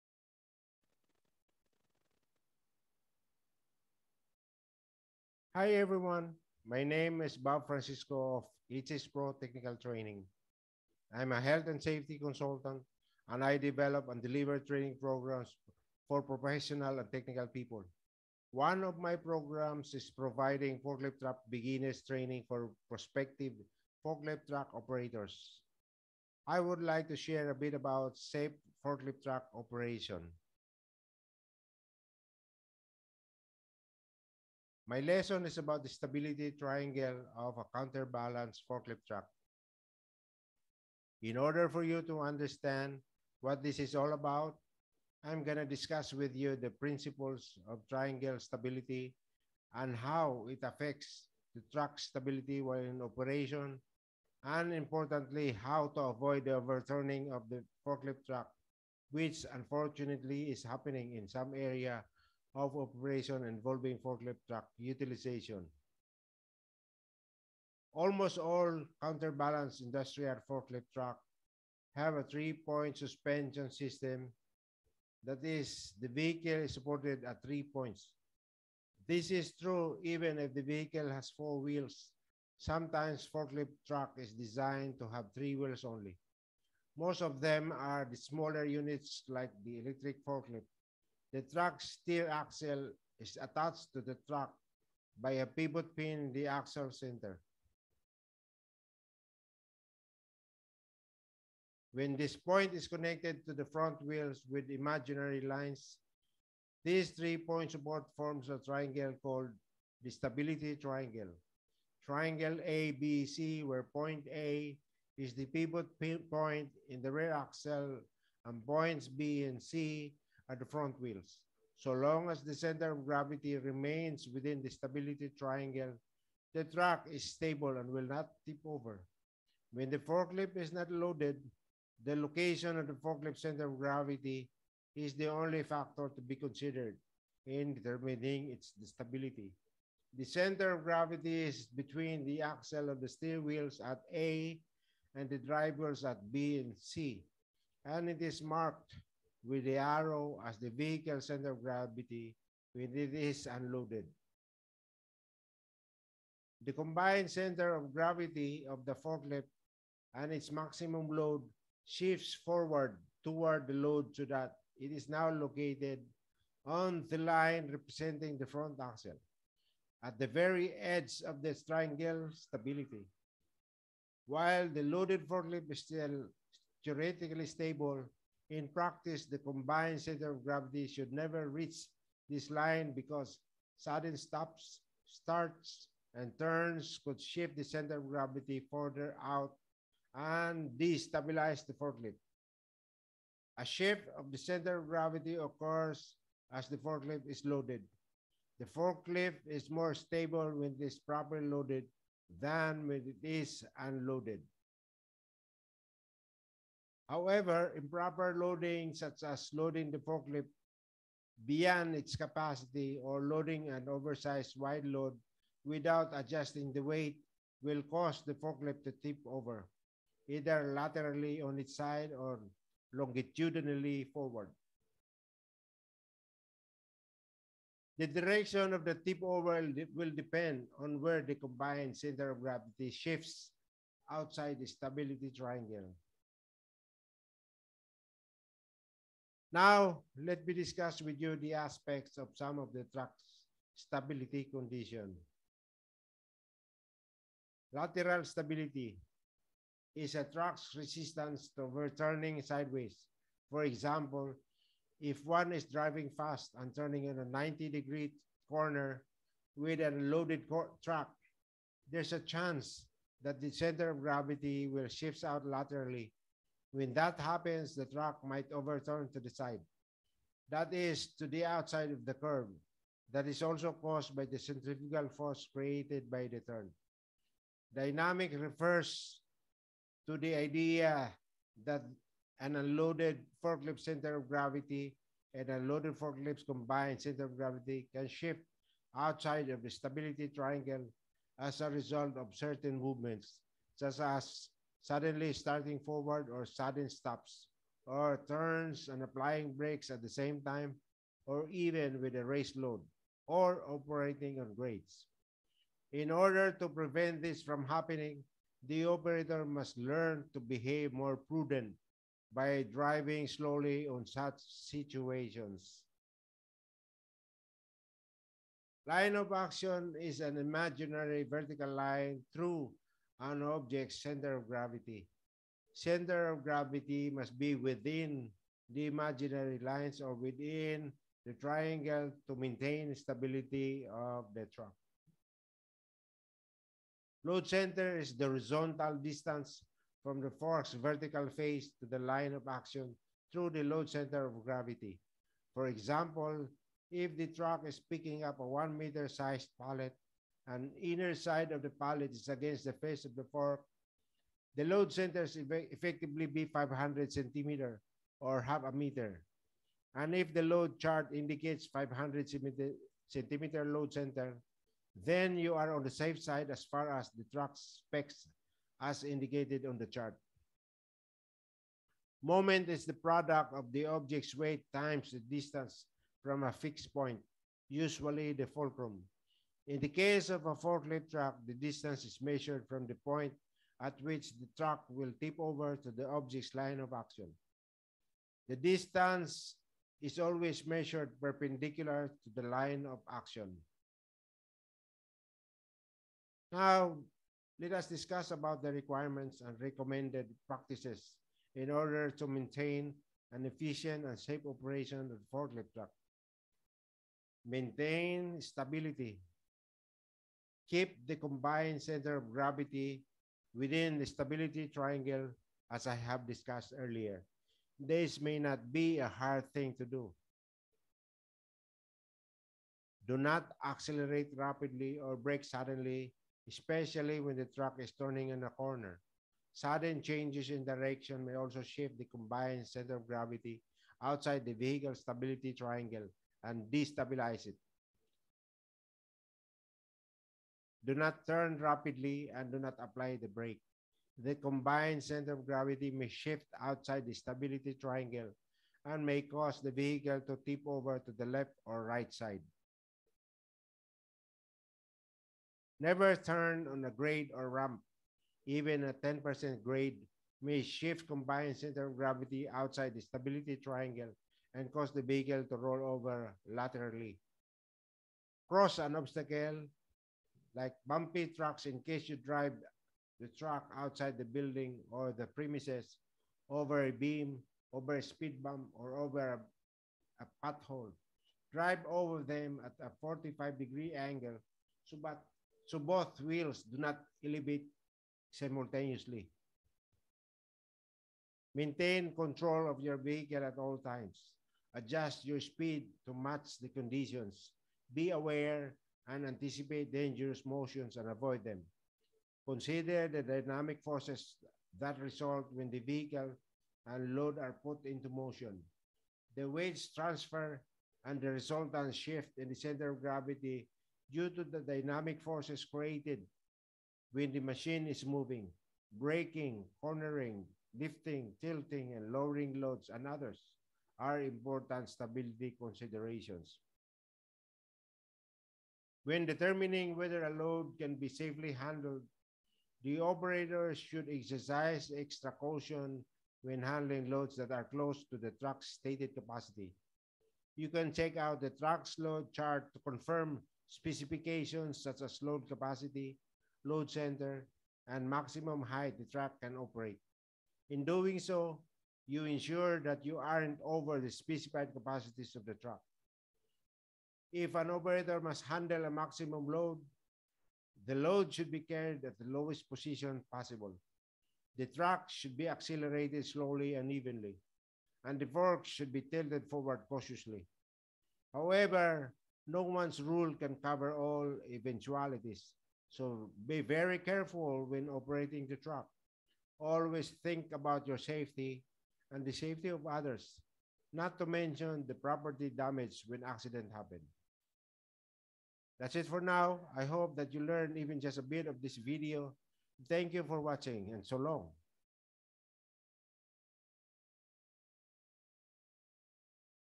Forklift Training What is Stability Triangle of Forklift Truck?
This video is about Forklift Training What is Stability Triangle of Forklift Truck. I have discussed about the stability triangle principle of counter balanced forklift truck and how it affects the lateral and longitudinal stability of forklift in operation. This is to ensure tipping over of the truck is avoided and maintain safe operation of the truck. I hope this information would provide idea especially to new truck operators.

bob technical training
technical training
training and development
management training
driving training
forklift training
counterbalanced truck